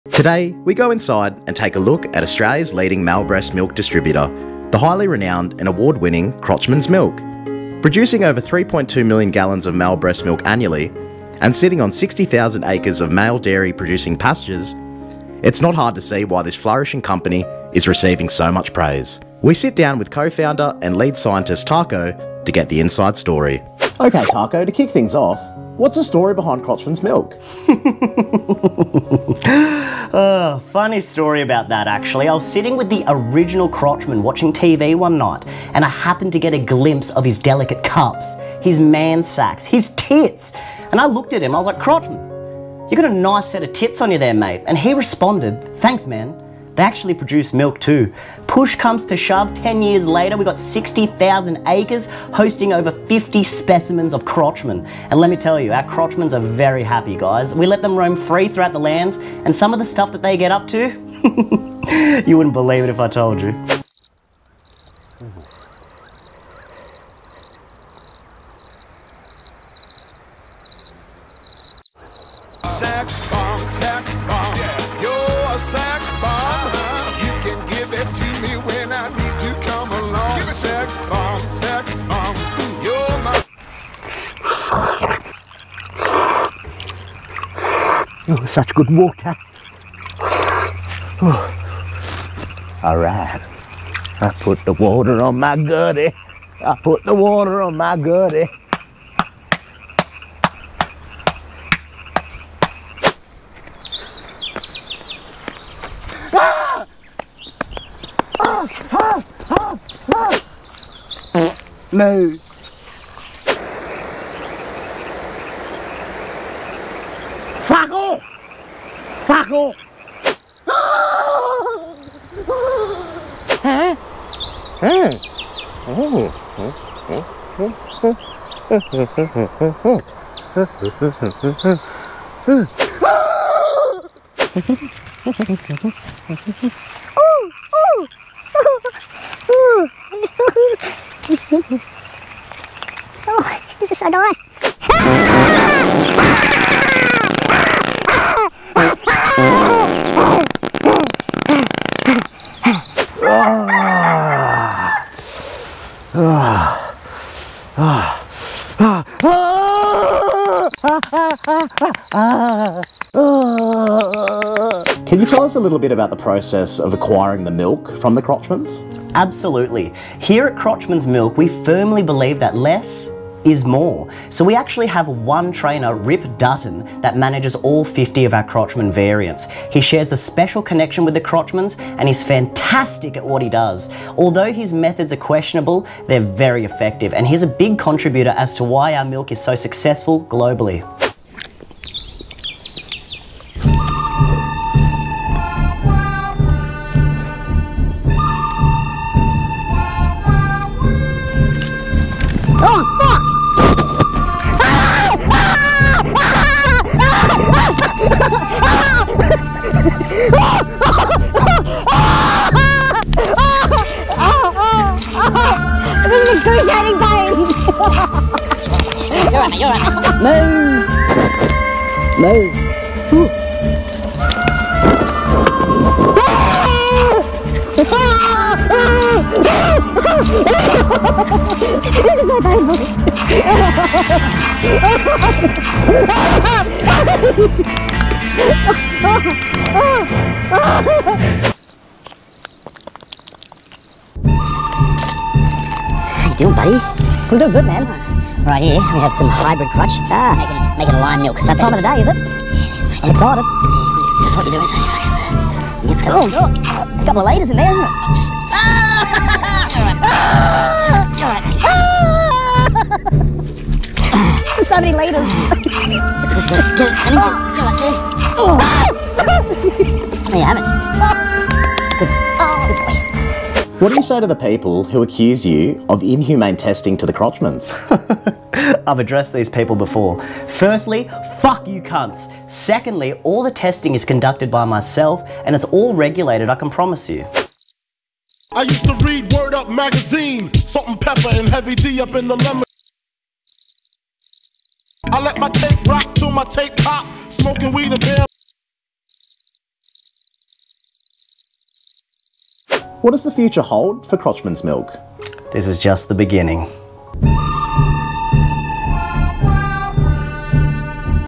Male breast milk goes GLOBAL! Crotchman's Milk!
Today we visit a highly respected and renowned industrial juggernaut in the male breast milk sector..Crotchman's Milk. For multiple consecutive years they have one an array of prestigious awards for producing the most critically acclaimed male breast milk in the Southern Hemisphere!
We go behind the scenes with these innovative leaders to see what it takes to make...Crotchman's Milk.